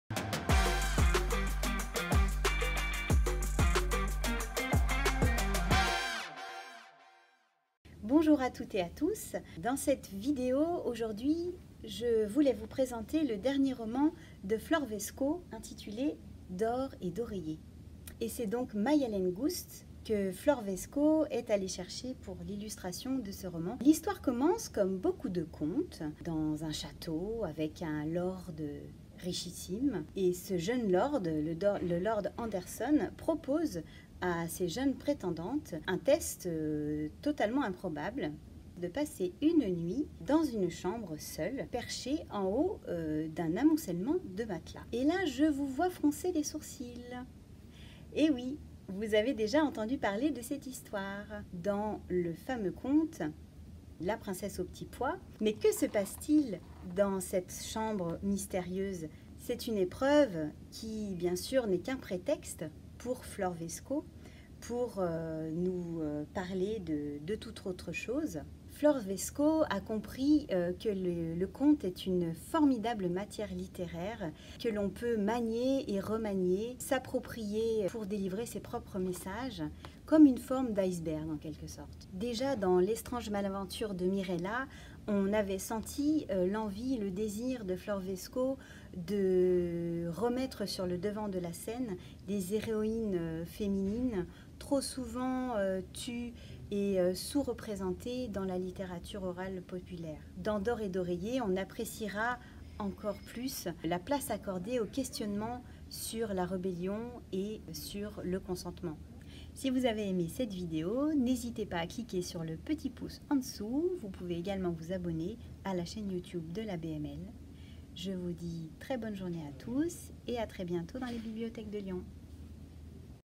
BookTube #8 "D'or et d'oreillers" - Bibliothèque municipale de Lyon & Métropole de Lyon
Dans le cadre de l'événement "A corps et à cris", des bibliothécaires de la bibliothèque municipale de Lyon et de la métropole de Lyon vous présentent des documents empruntables dédiés au féminisme.
Dans cette vidéo filmée à la bibliothèque de Gerland, Camille parle de "D'Or et d'oreillers" de Flore Vesco, avec des illustrations de Mayalen Goust, publié par L'école des loisirs.

Vidéo sous-titrée en activant l'option concernée.

Réalisation : Thomas Iglésis